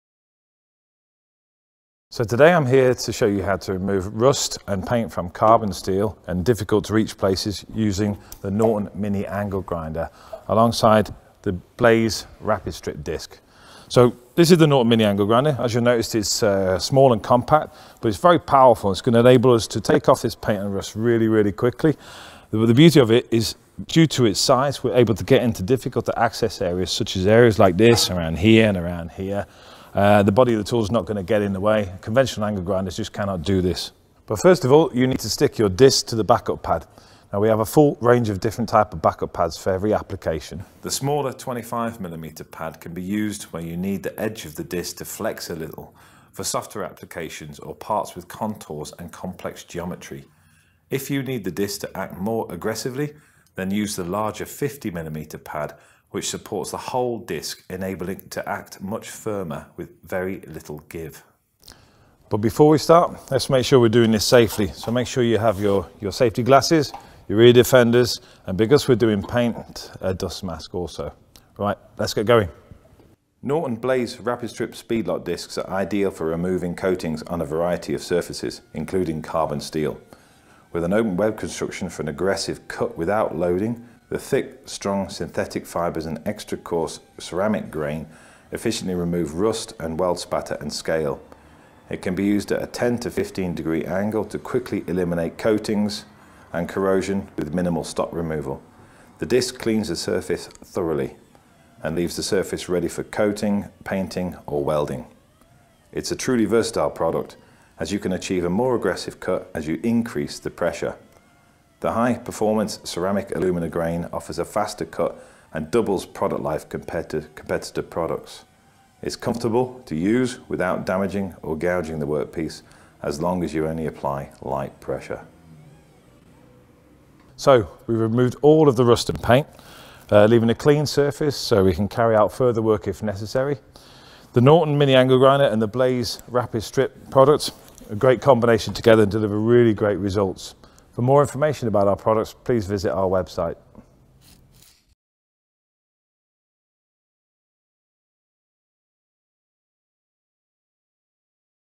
How to remove paint from metal with the Norton Mini Angle Grinder
Paul demonstrates how to quickly remove paint from metal using the Norton mini angle grinder, Norton Blaze Rapid Strip SpeedLok discs and this piece of painted carbon steel.
In no time, the awkward shaped surface of the painted metal is stripped clean and ready for coating or welding.